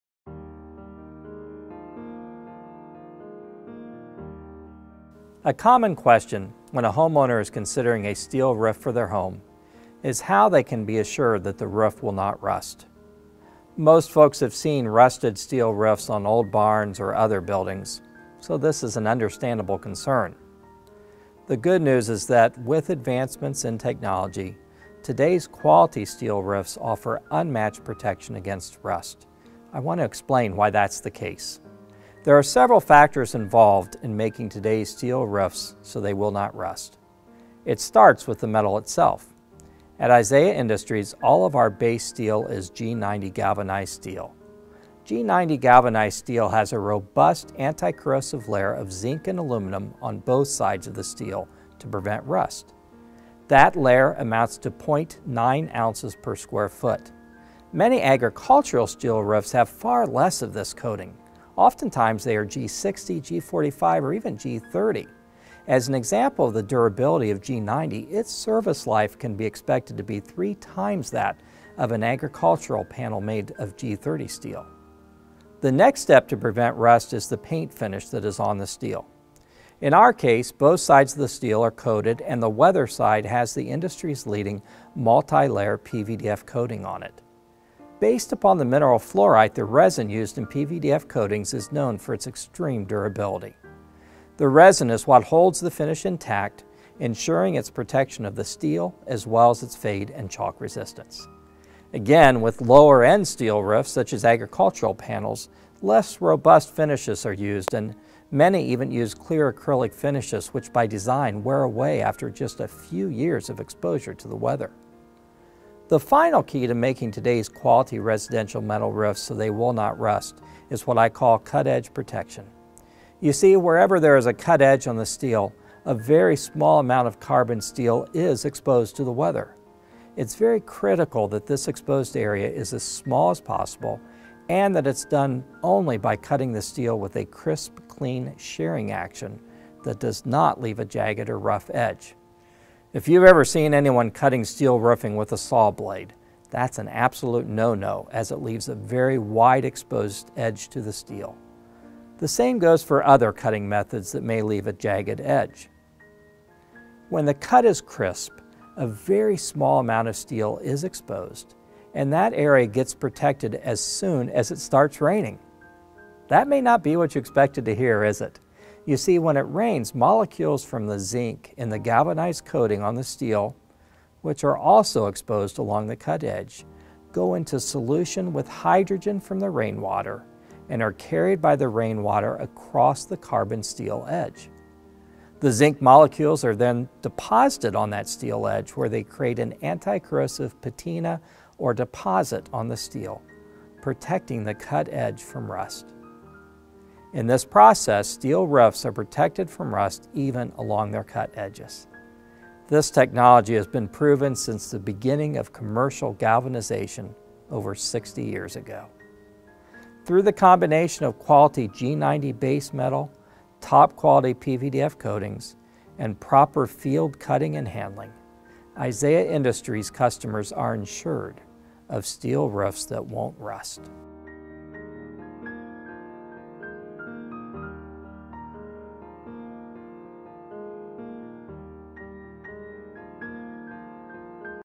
Steel Roofing and Rust Protection – Metal Roofing 101 with Todd Miller
A common question when a homeowner is considering a steel roof for their home is how they can be assured that the roof will not rust. Most folks have seen rusted steel roofs on old barns or other buildings, so this is an understandable concern. The good news is that, with advancements in technology, today’s quality steel roofs offer unmatched protection against rust.  I want to explain why that is the case.

There are several factors involved in making today’s steel roofs so they will not rust. It starts with the metal itself. At Isaiah Industries, all of our base steel is G90 galvanized steel. G90 galvanized steel has a robust anti-corrosive layer of zinc and aluminum on both sides of the steel to prevent rust. That layer amounts to 0.90 ounces per square foot. Many agricultural steel roofs have far less of this coating. Oftentimes they are G60, G45, or G30. As an example of the durability of G90, its service life can be expected to be three times that of an agricultural panel made from G30 steel.

The next step to prevent rust is the paint finish that is on the steel. In our case, both sides of the steel are coated and the weather side has the industry’s leading multi-layer PVDF coating. Based on the mineral fluorite, the resin used in the PVDF coatings is known for its extreme durability. The resin is what holds the finish intact, ensuring its protection of the steel as well as its fade and chalk resistance. Again, with lower end steel roofs such as agricultural panels, less robust finishes are used and many even use clear acrylic finishes which, by design, wear away after just a few years of exposure to the elements.

The final key to making today’s quality residential metal roofs so they will not rust is what I call cut edge protection. You see, wherever there is a cut edge on the steel, a very small amount of carbon steel is exposed to the weather. It’s very critical that this exposed area is as small as possible and that is done by only cutting the steel with a crisp, clean shearing action that does not leave a jagged or rough edge. If you’ve ever seen anyone cutting steel roofing with a saw blade, that is an absolute no-no as it leaves a very wide exposed edge to the steel. The same goes for other cutting methods that may leave a jagged edge.

When the cut is crisp, a very small amount of steel is exposed and that area gets protected as soon as it starts raining. That may not be what you expected to hear, is it? You see, when it rains, molecules from the zinc in the galvanized coating on the steel, which are also exposed along the cut edge, go into solution with hydrogen from the rain water and are carried by the rain water across the carbon steel edge. The zinc molecules are then deposited on the steel edge where they create an anti-corrosive patina or deposit on the steel, protecting the cut edge from rust.

In this process, steel roofs are protected from rust even along their cut edges. This technology has been proven since the beginning of commercial galvanization over 60 years ago. Through the combination of quality G90 base metal, top quality PVDF coatings, and proper field cutting and handling, Isaiah Industries customers are ensured of steel roofs that won’t rust.